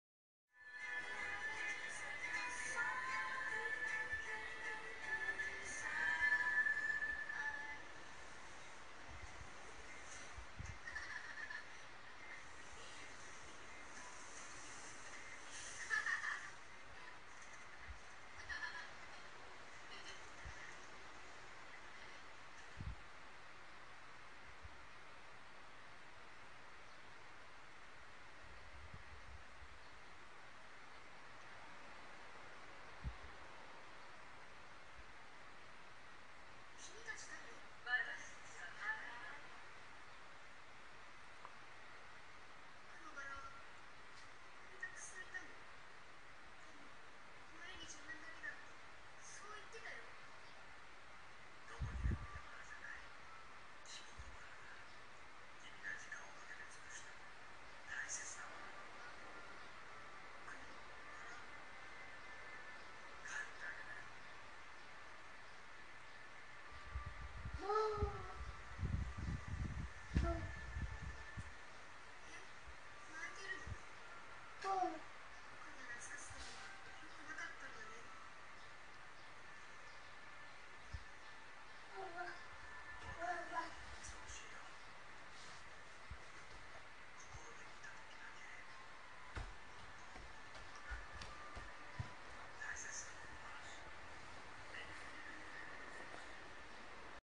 僕とＧＷとお母さん
お母さんがいるのに僕はDVDに夢中で、可哀想なお母さんはその背中を録画するって言う…。